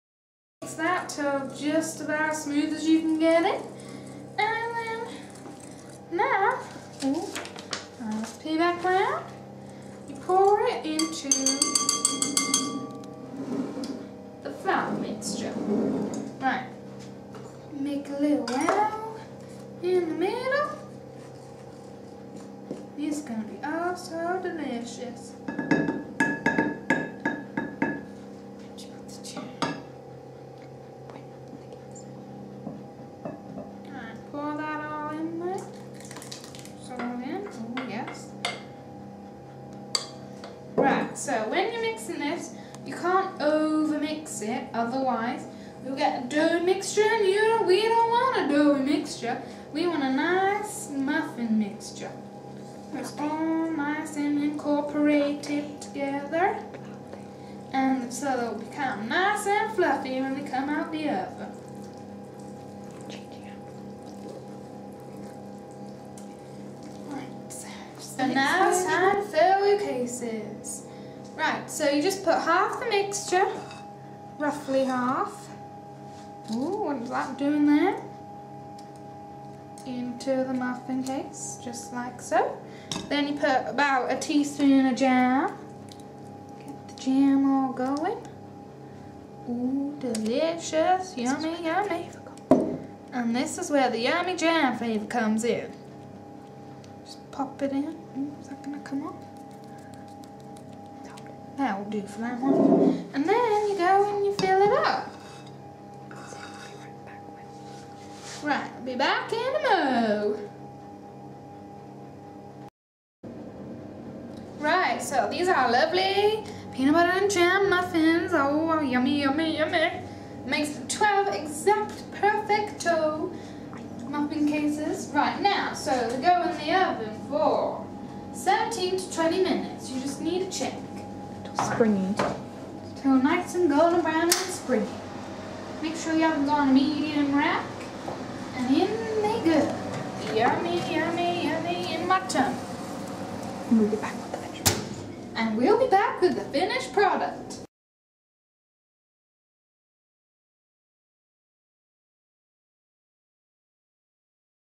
How To Bake jelly and peanut butter muffins
Delicious Muffins !
I know we are quite quiet in this video but if you look at our previous videos you will find we show even more personality